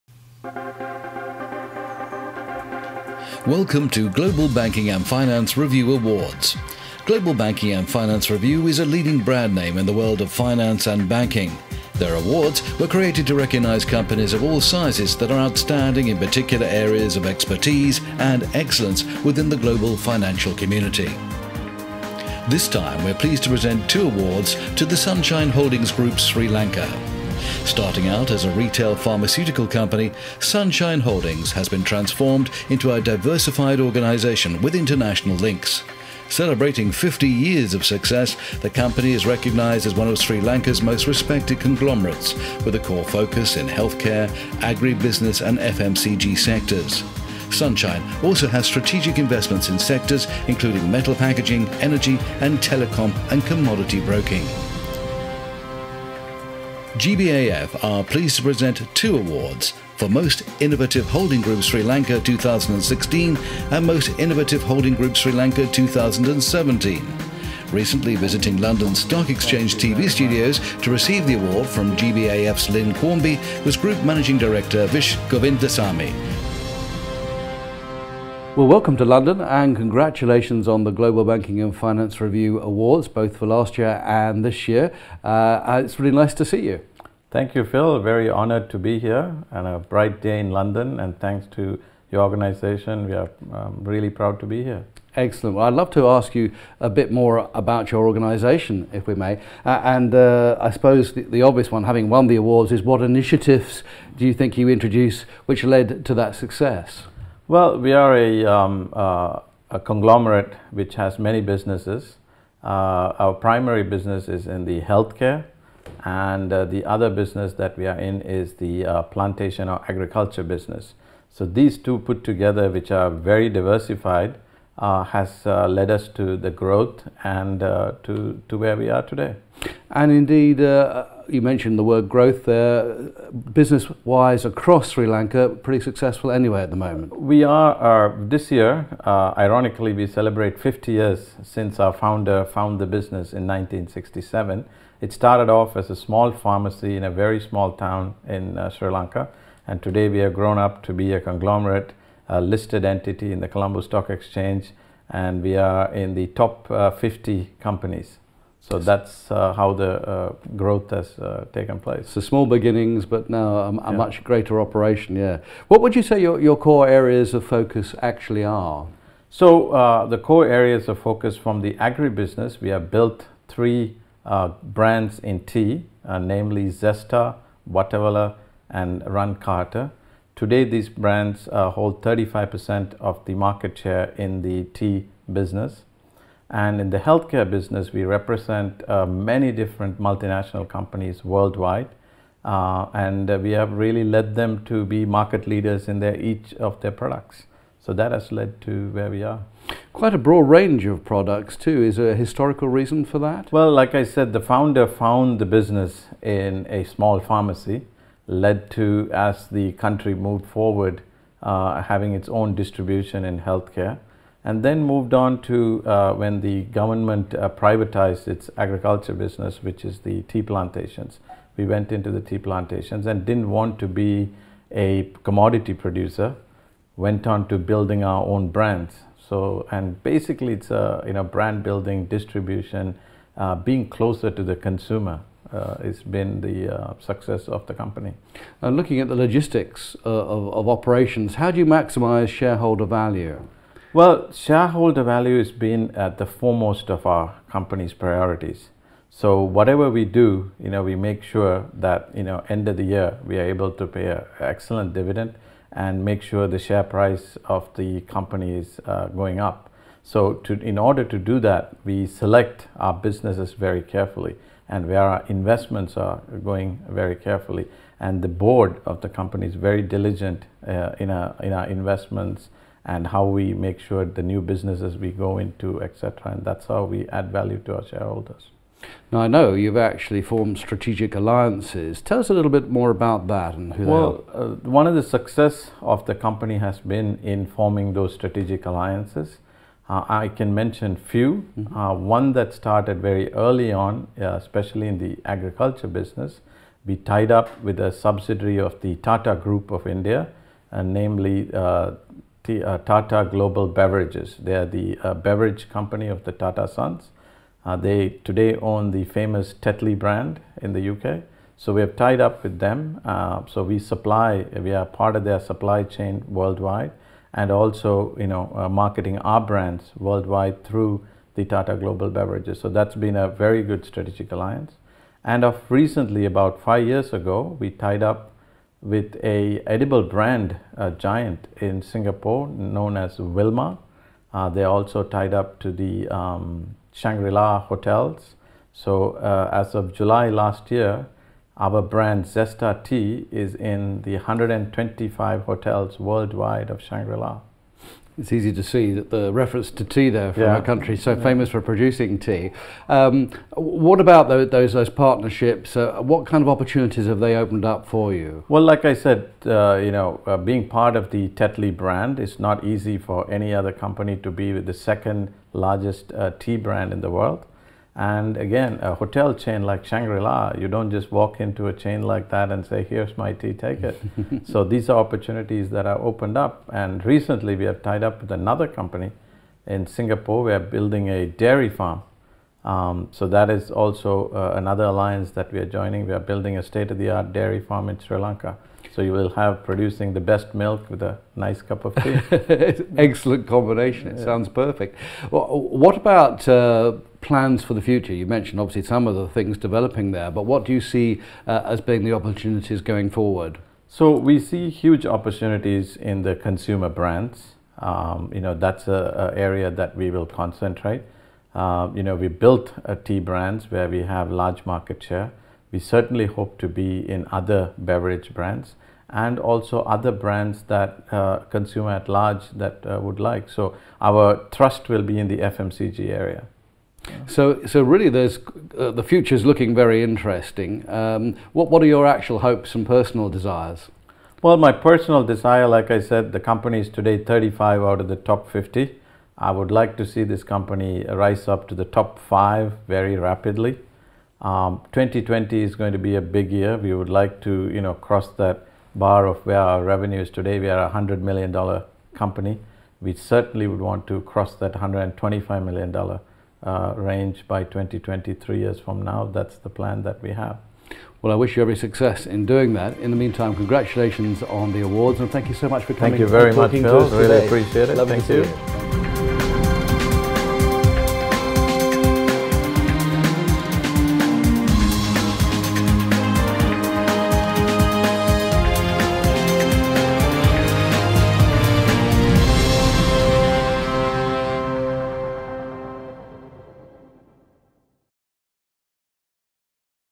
Celebrating 50 years of Successful Business
Global Banking & Finance Review interview Sunshine Holding, Group Managing Director, Mr. Vish Govindasamy